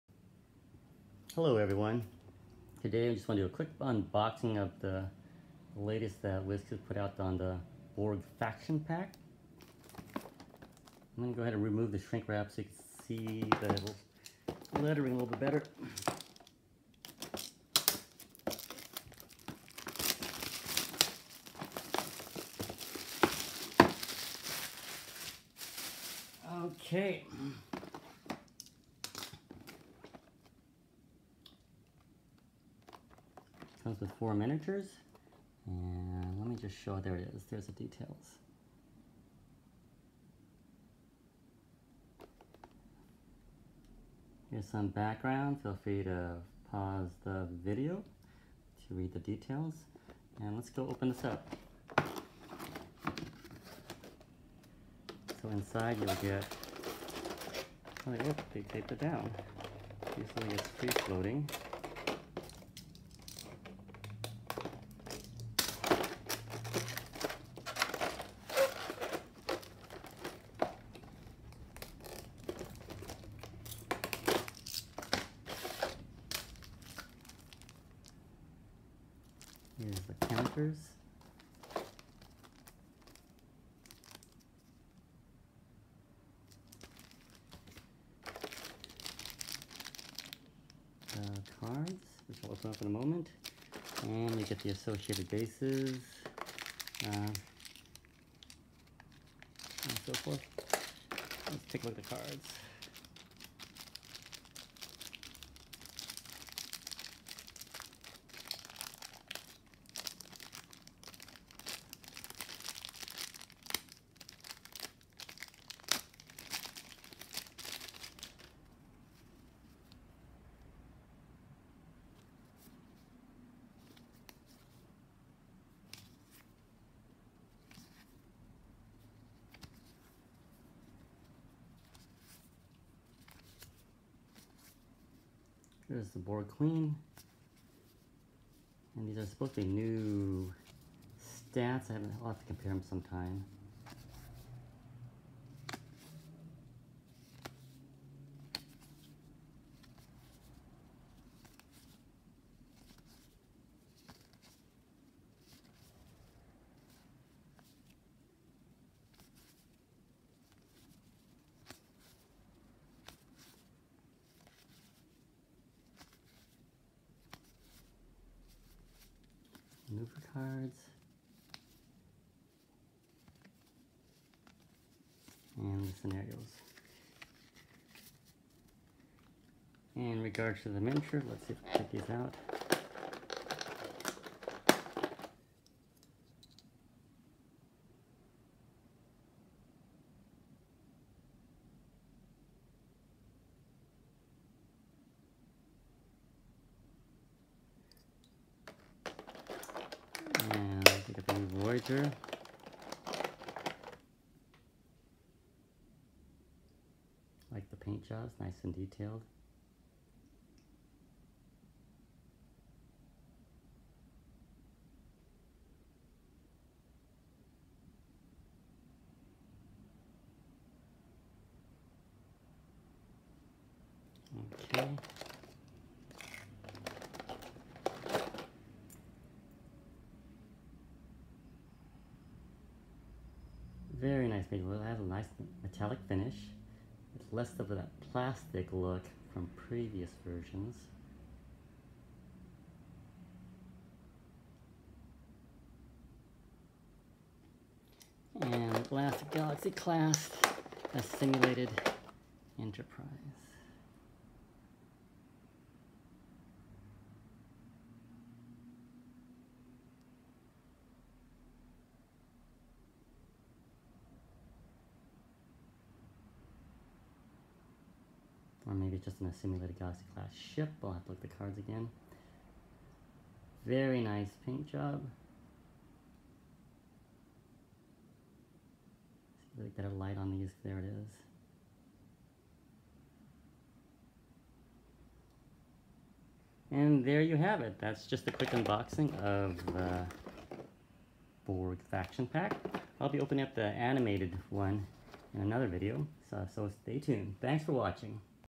Star Trek Attack Wing Borg Faction pack - Unboxing
Quick video on the new Borg Faction Pack recently released by Wizkids.